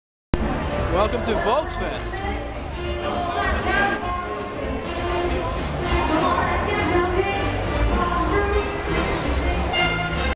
Volks Fest PART 1
Exchange Students at Volksfest, with leaders.